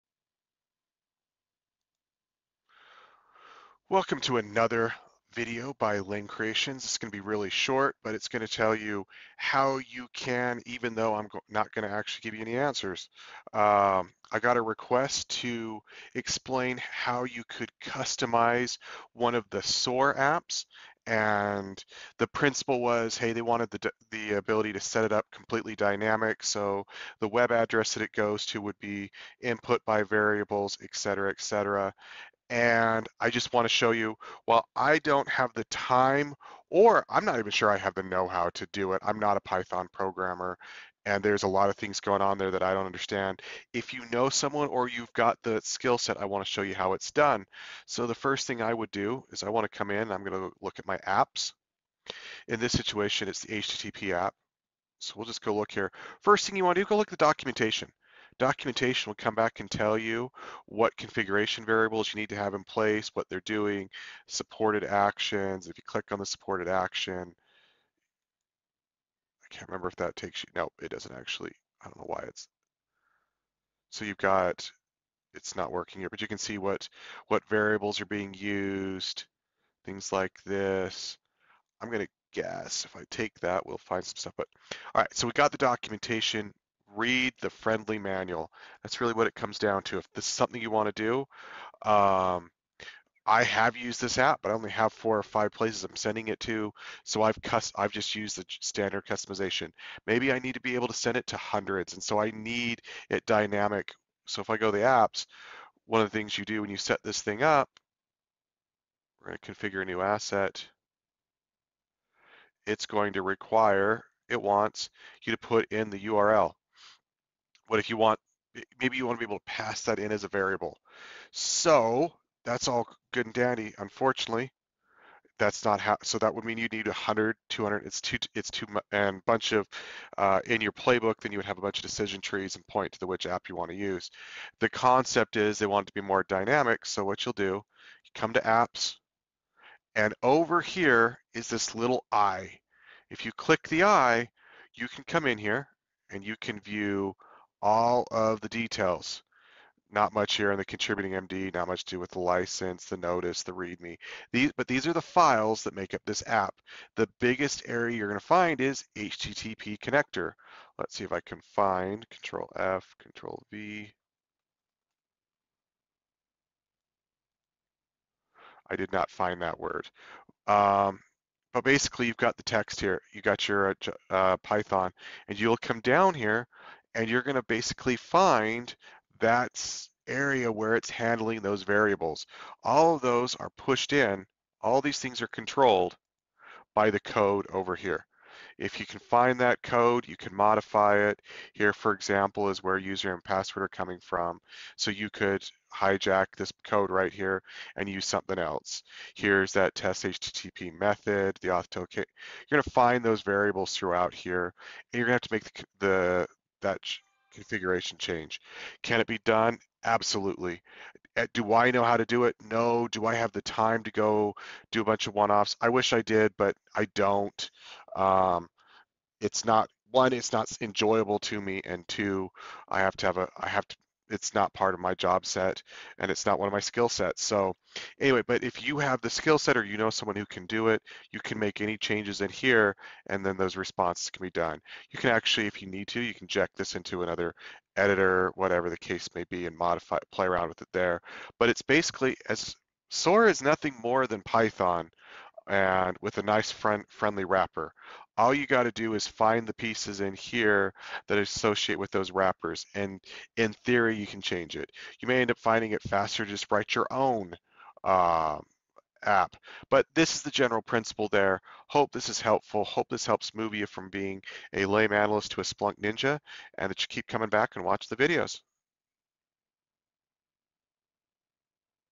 Intro to Splunk SOAR Phantom | How to Customize a SOAR App
Playlist Link for SOAR Videos

This is the a continuation of a video playlist  dedicated to understanding how to set up and leverage Splunk SOAR / Phantom.  This video will show where the documentation for the app and where you go to see the source code to modify for the app can be located.